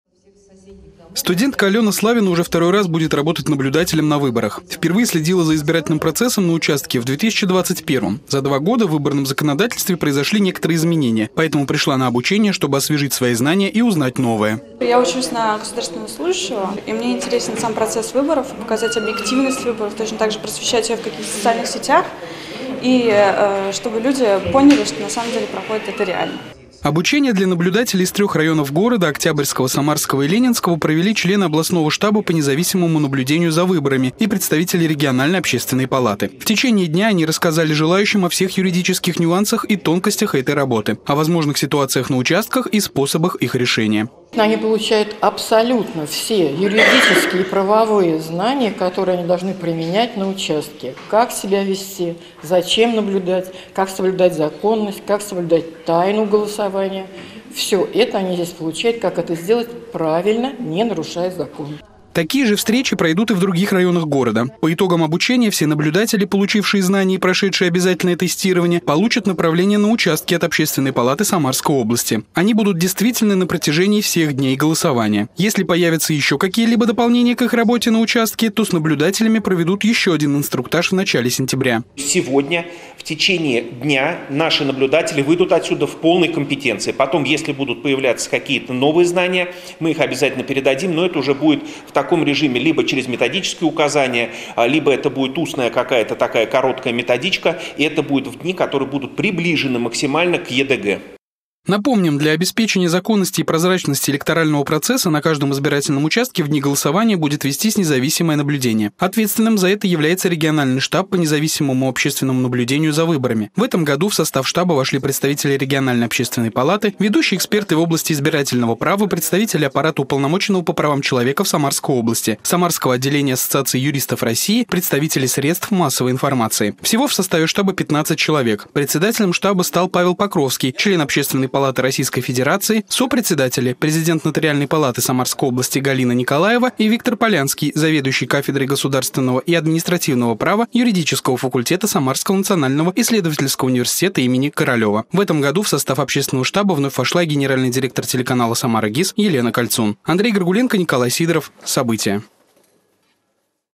В Самарской области продолжается обучение независимых наблюдателей за выборами
В администрации Октябрьского района Самары прошло обучение независимых наблюдателей за выборами. Слушали лекции и выполняли разные практические задания жители трех районов - Октябрьского, Самарского и Ленинского. Однодневное обучение провели специалисты областного штаба независимого наблюдения за выборами. Какой документ получат активные граждане, расскажем в нашем сюжете.